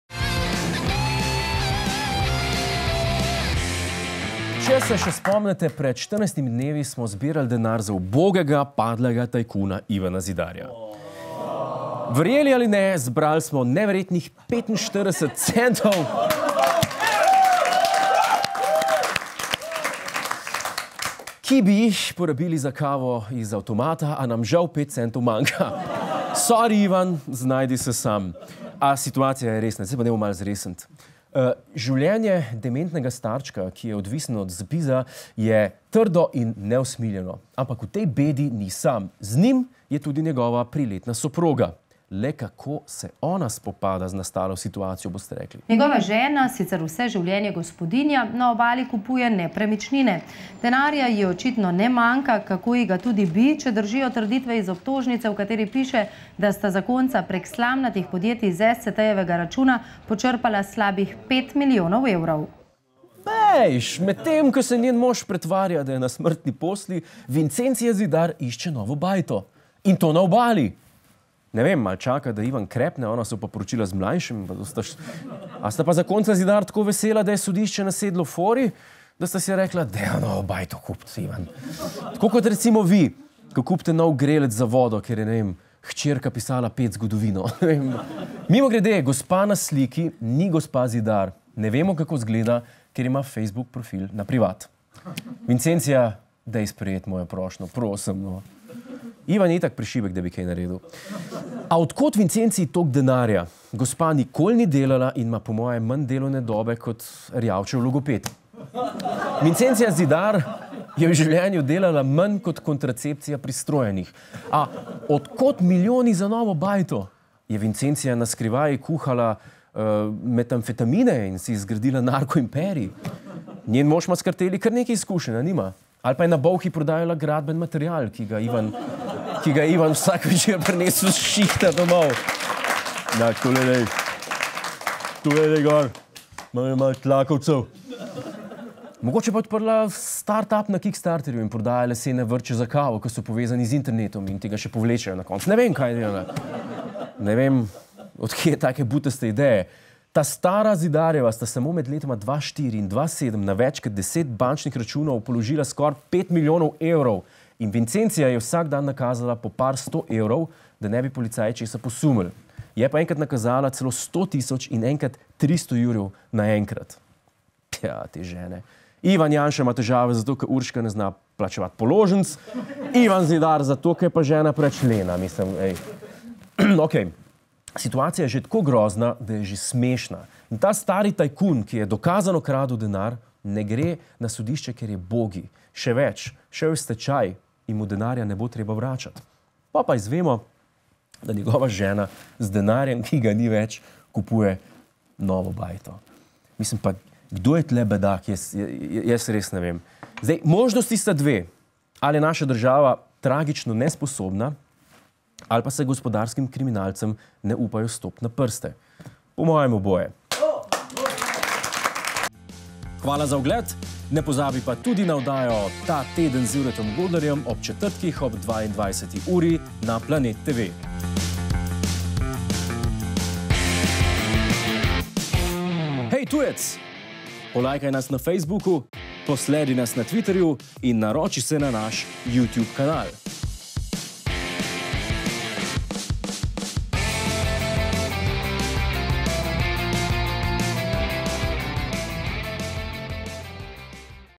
Ta Teden: Zidar in Vincencija
Zidar in Vincencija sta lisjaka dva, ki marsikaj počenjata.

Deli video s sodržavljani Republike Slovenije!